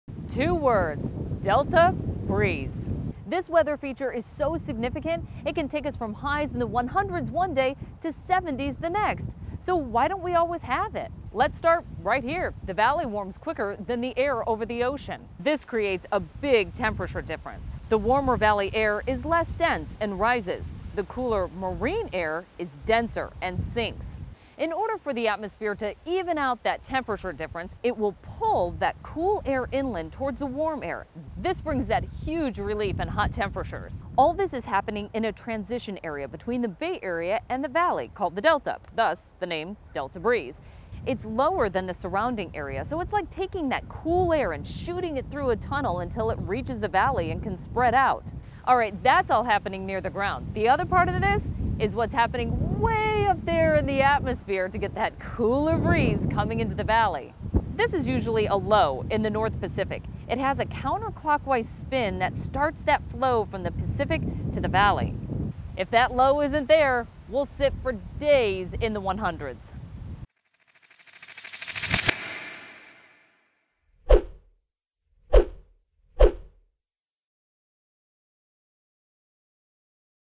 What is the Delta Breeze? | Geek Lab
The Delta Breeze can bring big relief to summer heat in the Sacramento area. Here's why we sometimes get this cooling wind from the Pacific.